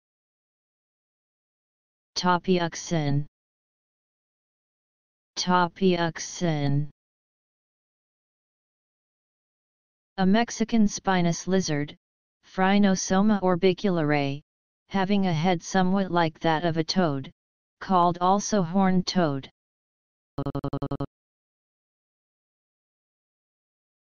Tapayaxin Meaning
A Mexican spinous lizard (Phrynosoma orbiculare) having a head somewhat like that of a toad; -- called also horned toad.

In this video, you will learn "Tapayaxin" Meaning and pronunciation in English.

Wordy Affair